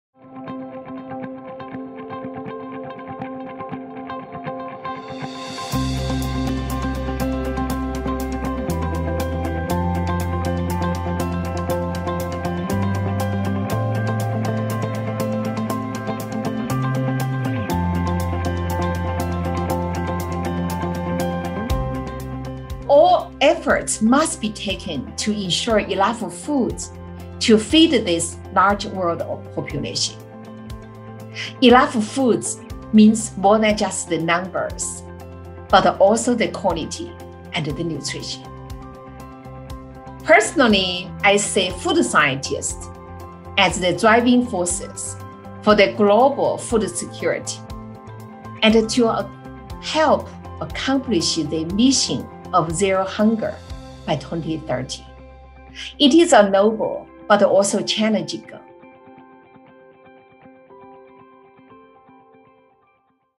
2021 Achievements in Microbial Research for Food Safety recipient on eradicating global hunger
What will it take to eradicate global hunger and malnutrition?

Yaguang Luo, PhD, the 2021 recipient of the Achievements in Microbial Research for Food Safety in honor of Gerhard J. Haas, shares her perspective.